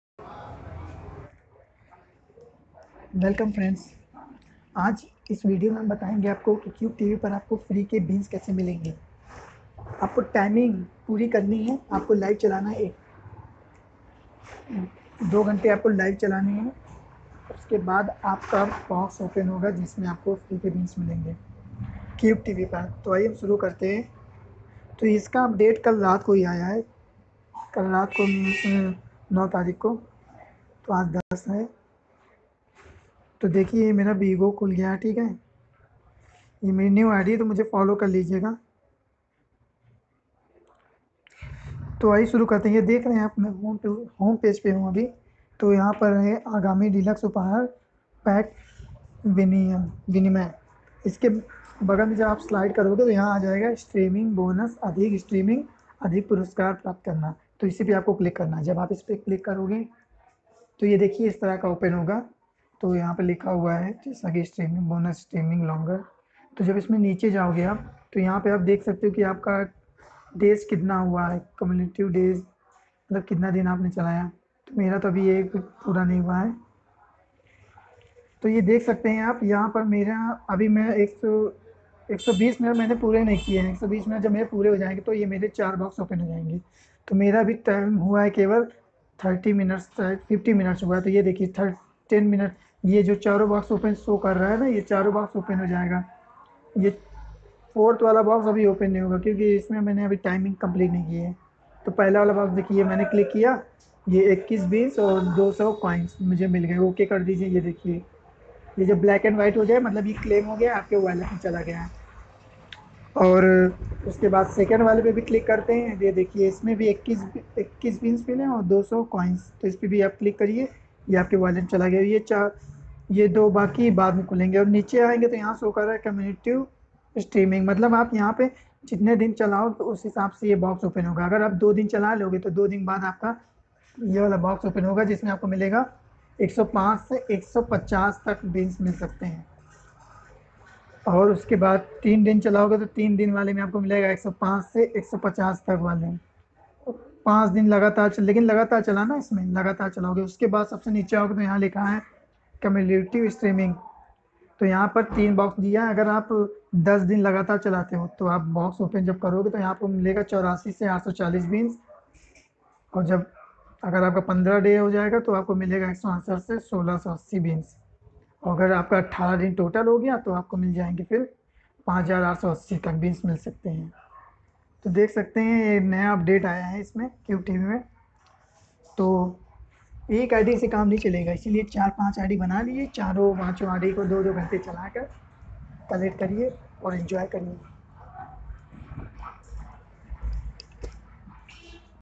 Cube tv has stopped daily reward from 1 september
Friends 31 August se reward band ho jyega box open nhi ho rha ab
 Cubetv is not working in india now